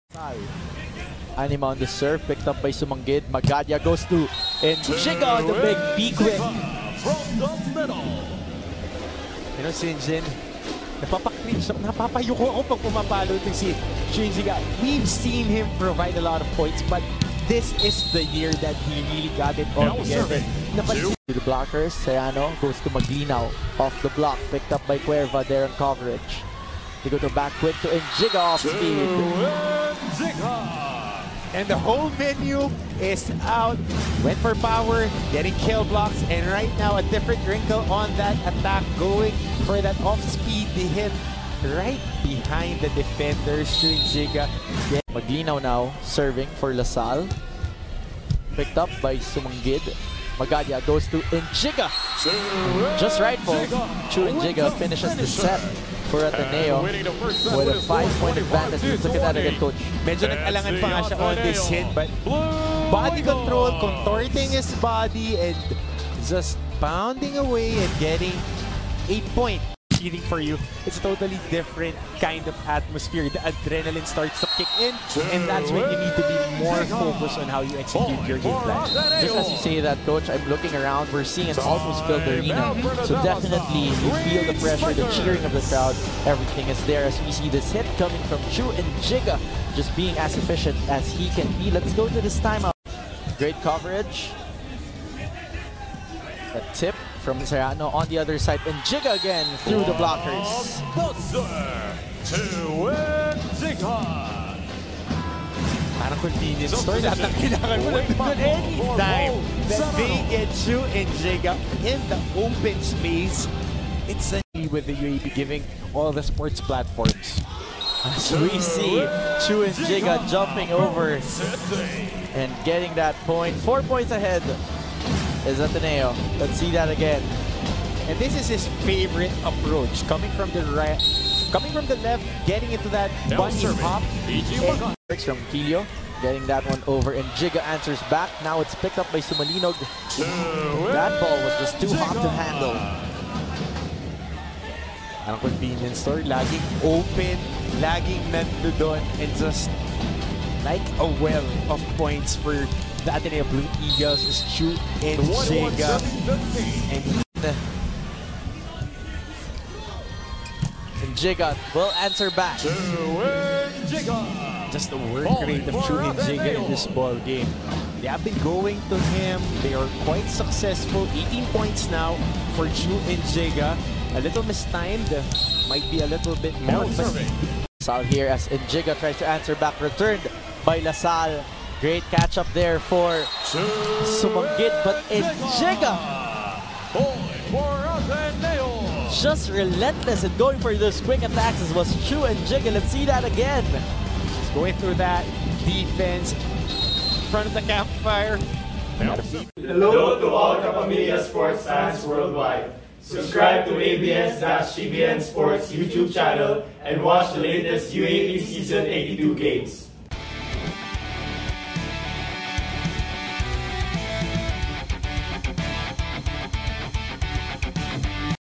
KING EAGLE? Chu Njigha catches FIRE for 20 pts against La Salle | UAAP 82 MV
Ateneo middle Chu Njigha makes his presence felt against La Salle, dropping 19 BIG points to tow the Blue Eagles to their first win.